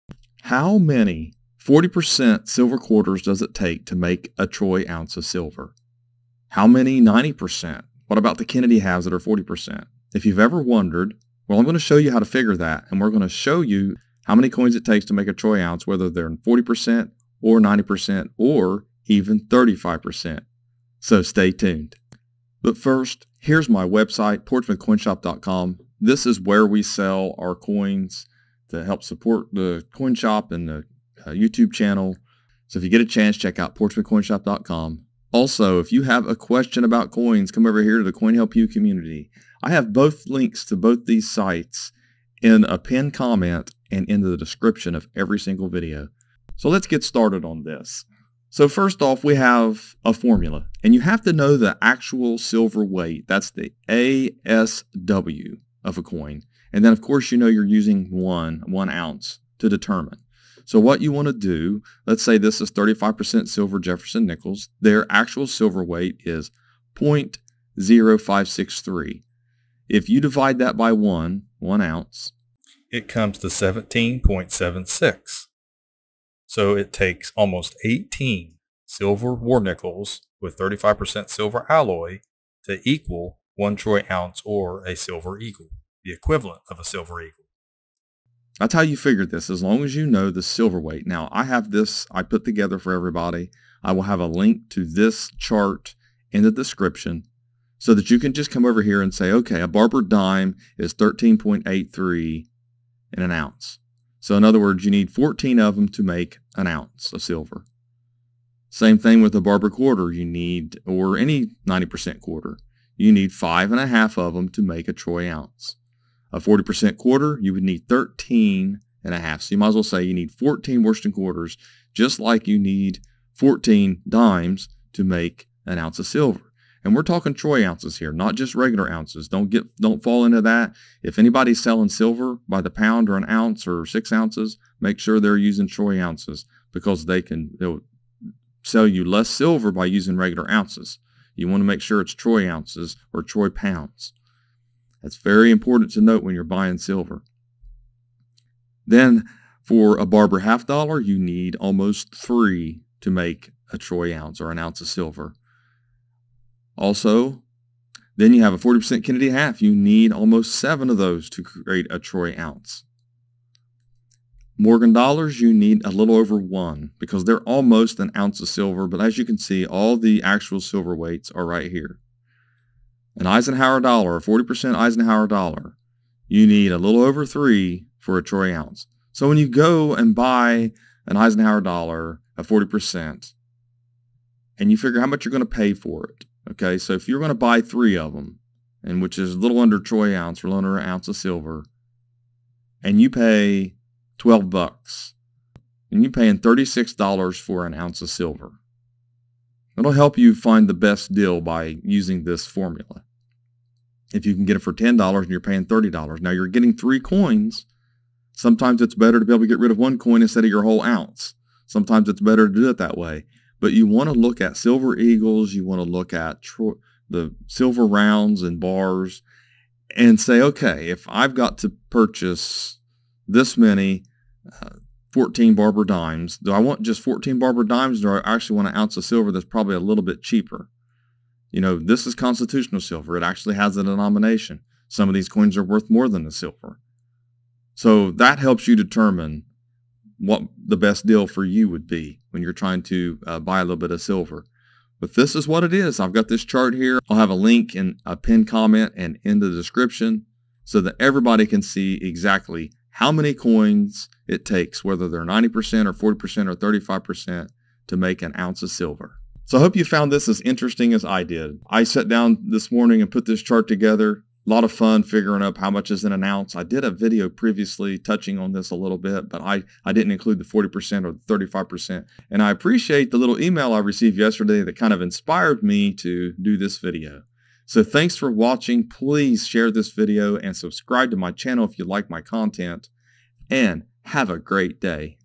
How Many War Nickels To Make A Silver Eagle? Junk Silver Math
How Many War Nickels To Make A Silver Eagle? What About 90%?
In this video we show you how to determine how many 90% silver coins, 40% silver coins and 35% silver coins it takes to make an ounce of silver.
How Many 40% Silver Coins, 35% Silver Coins Or 90% Silver Coins In An Ounce?

It takes approximately 19, 35% silver Jefferson war nickels to make an ounce of silver.

It takes approximately 14, 90% silver Barber or Roosevelt or Mercury dimes, to make an ounce of silver.

It takes approximately 14, 40% 1976 Bicentennial, Drummer Boy, quarters to make an ounce of silver.

It takes 5.5. 90% Barber or Standing Liberty or Washington quarters to make an ounce of silver.

It takes approximately 3, 90% Barber or Walking Liberty or Franklin or Kennedy half dollars to make an ounce of silver.

It takes 1.29, 90% Morgan or Peace Silver Dollars to make an ounce of silver.

It takes a little over 3, 40% Eisenhower Dollars to make an ounce of silver.